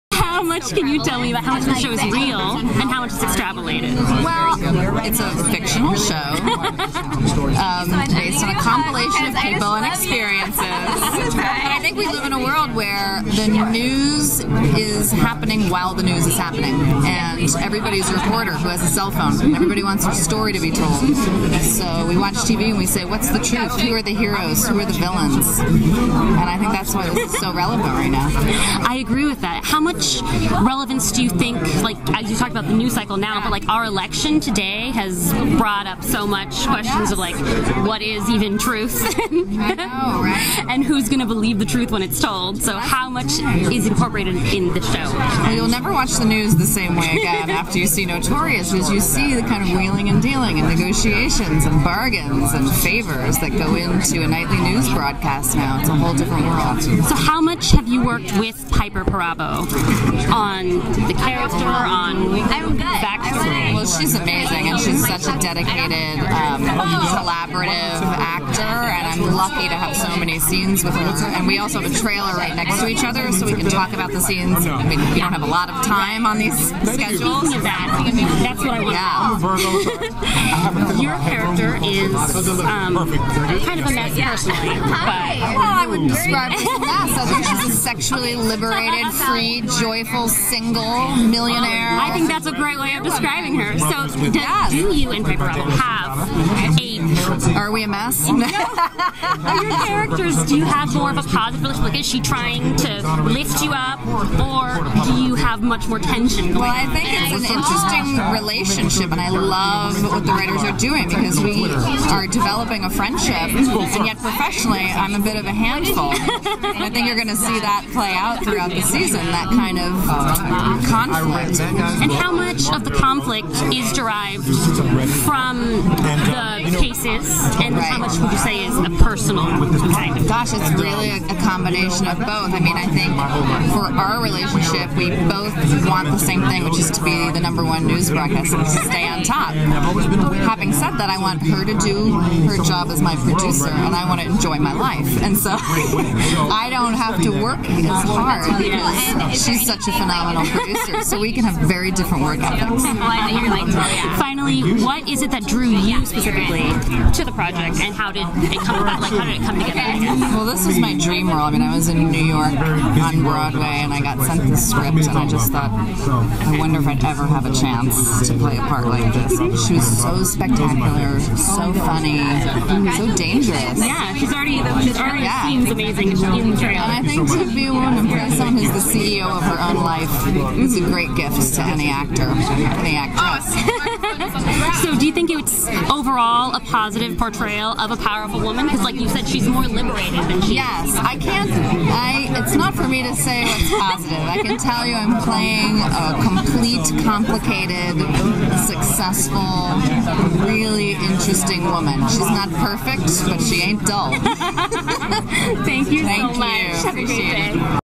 Paley Fall Preview 2016: Notorious Kate Jennings Grant on Playing a Handful!
Notorious actress Kate Jennings Grant (Louise Herrick) talked about her new respect for news broadcasting and her character's potential to be a mess!